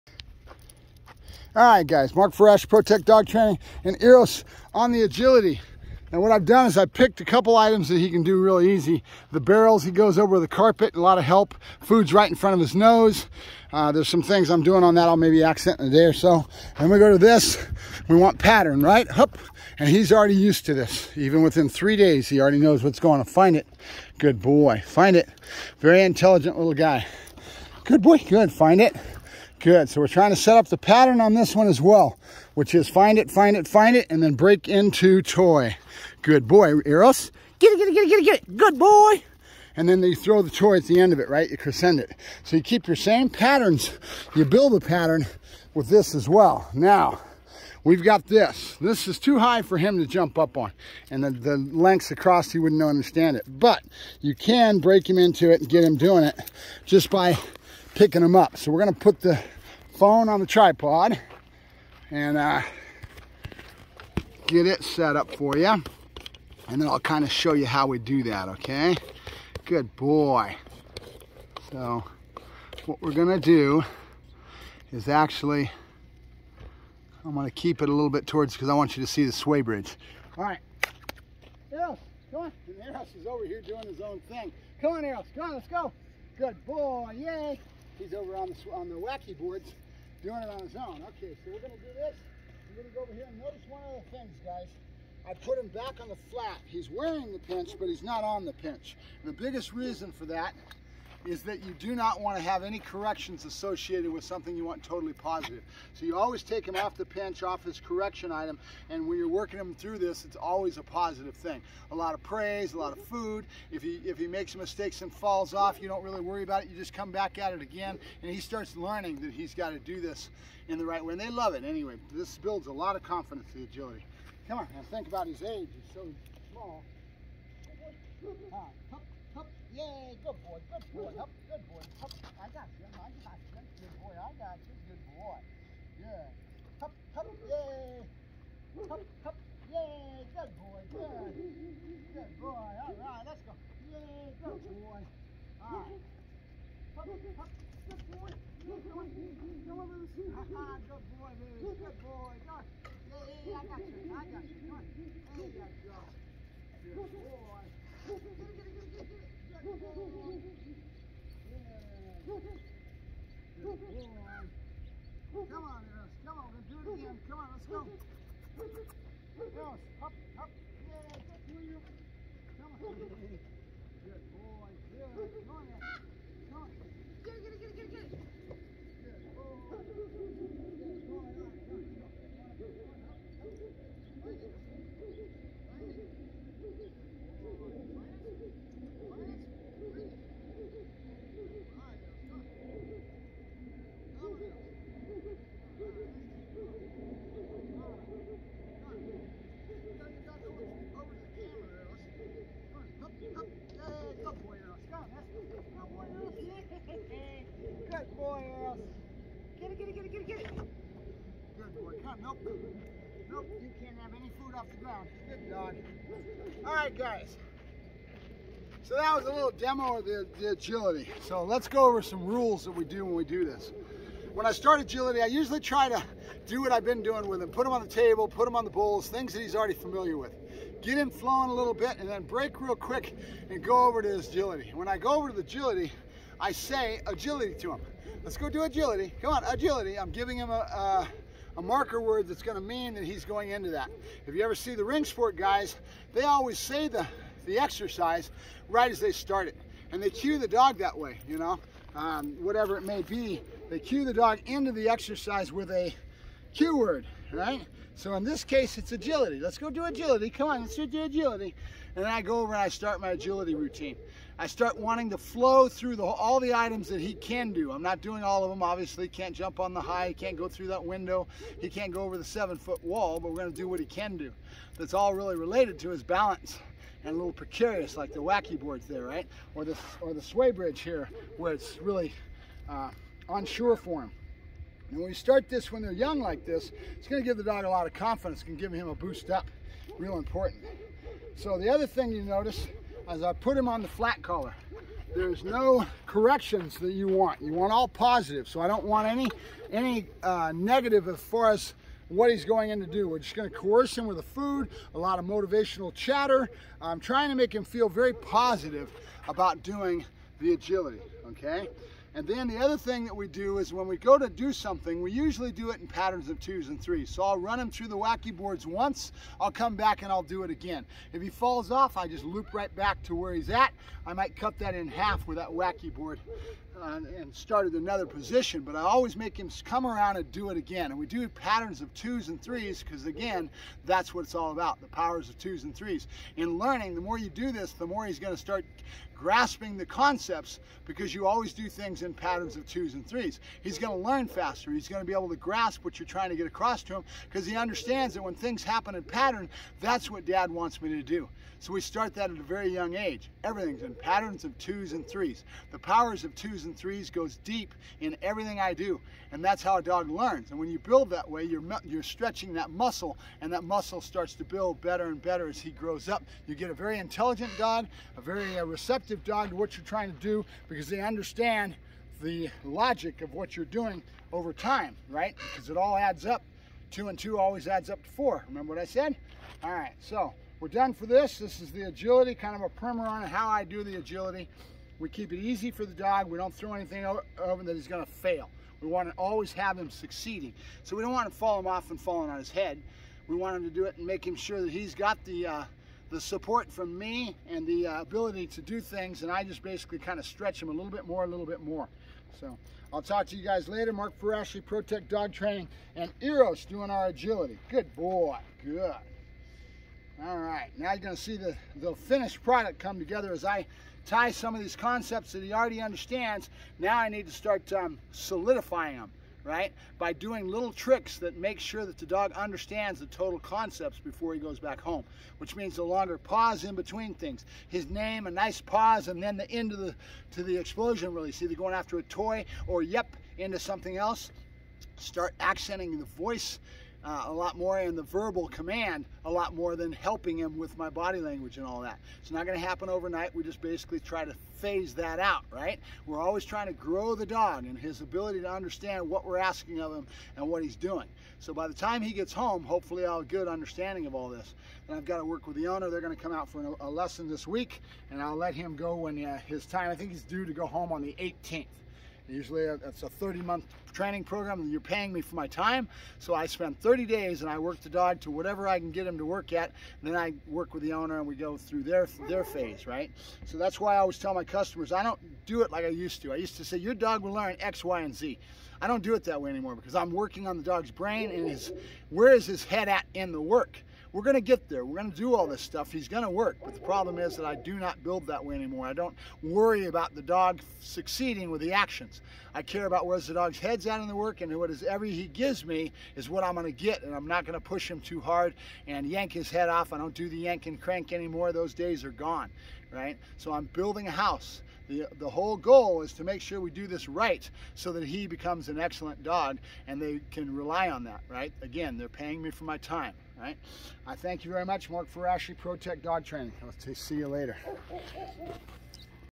Eros and agility
Mark Ferrasci w/Pro-Tech Dog Training. working Eros into his agility work. The main reason for the type of agility build I do with my dogs is to build confidence in the animal.